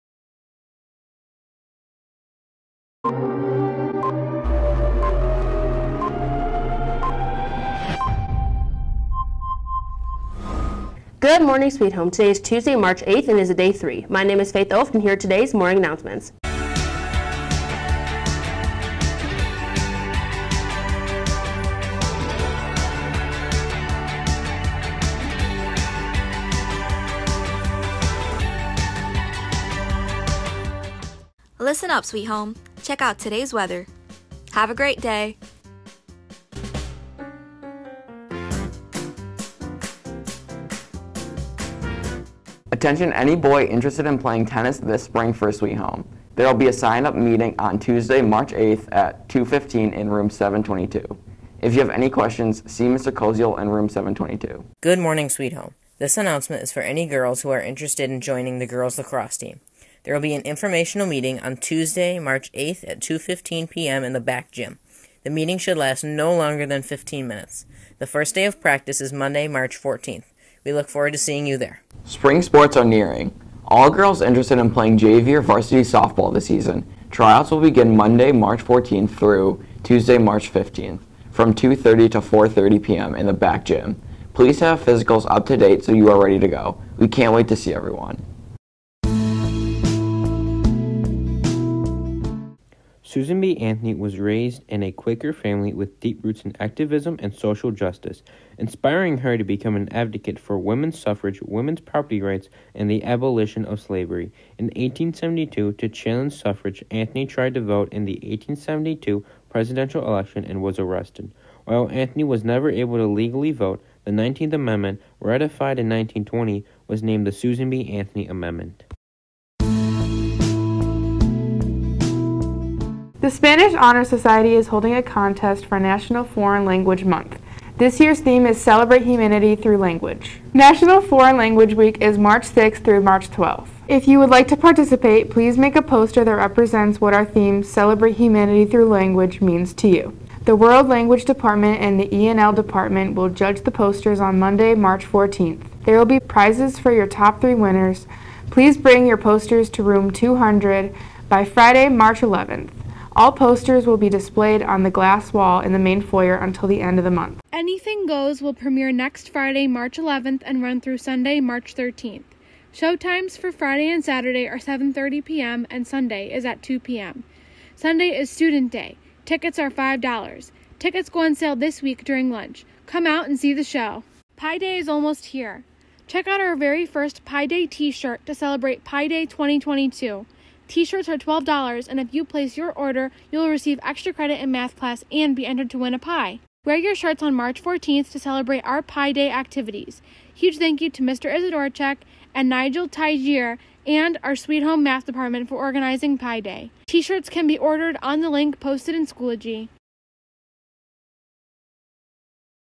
3/8 Morning Announcements
Sweet Home High School Morning Announcements

- Intro from Faith Ooft
- Lunch
- Weather
- Boys Tennis Sign Up Meeting - 3/8
- Girls Lacrosse Team Meeting
- Softball Tryouts
- Women’s History Month - Susan B. Anthony
- Spanish Honor Society - National Foreign Language Week - March 6-12
- Pi Day